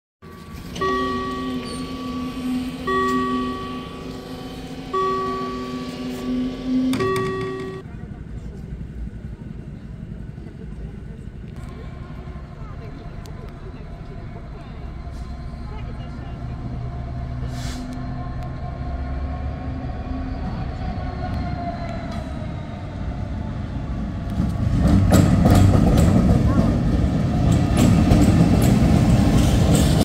hgk Köln Braunkohlestaub zug mit claas 66lok in Köln am Übergang Aachener Straße nach frechen
Hgk Köln 2024